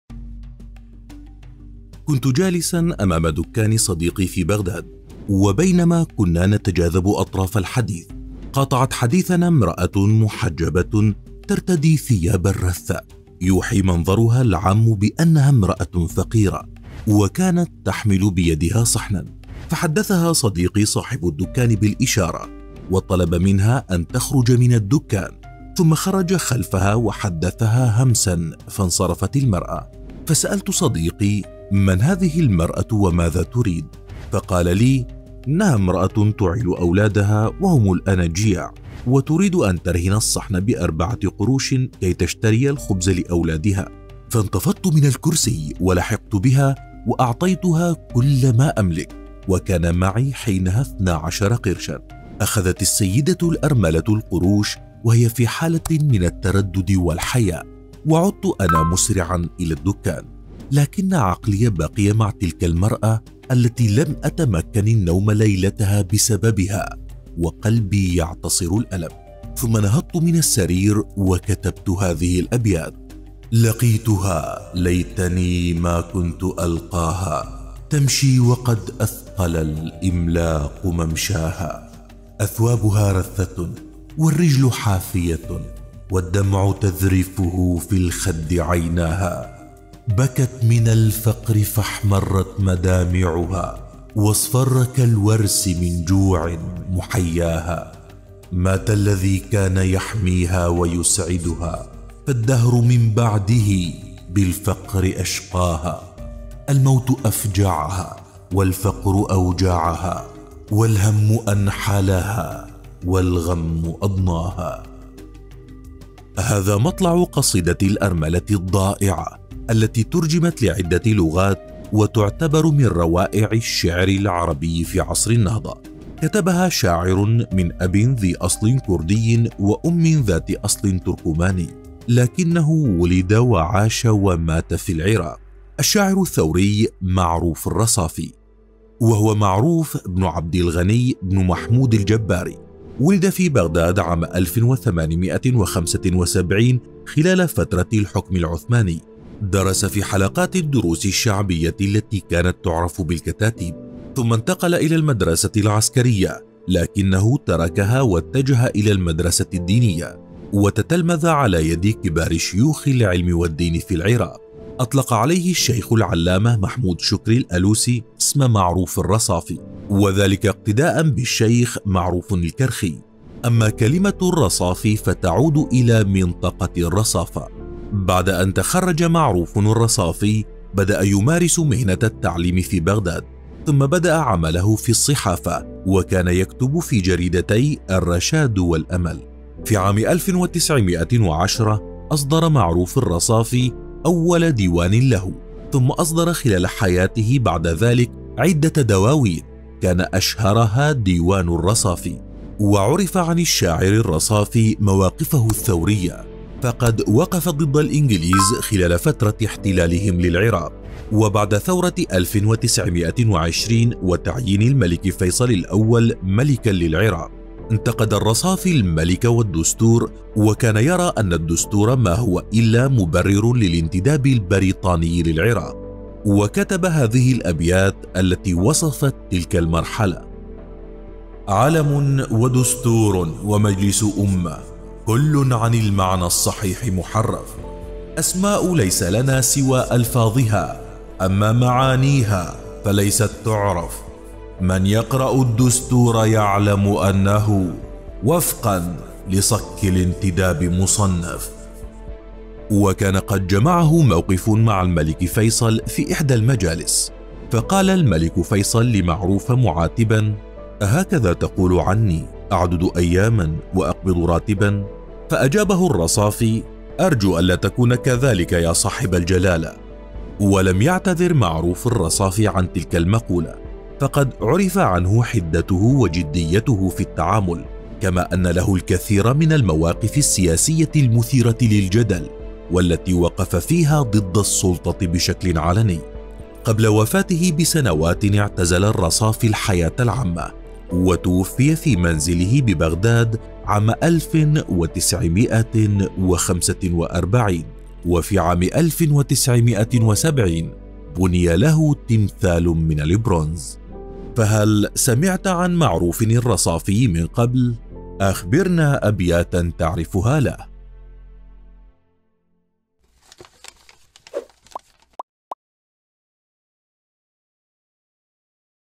الأرملة المرضعة قصيدة تصف حال العراق وترجمت لعدة لغات كتبها معروف الرصافي بدموعه
الأرملة المرضعة قصيدة تصف حال العراق كتبها معروف الرصافي بعد موقف جمعه مع امرأة أرملة فما هي هذه القصيدة ومن هو معروف الرصافي الذي كان عدواً للملك فيصل
00:00 الأرملة المرضعة
2:00 معروف الرصافي
3:30  الملك فيصل ومعروف

حساباتنا على السوشيال ميديا
تويتر: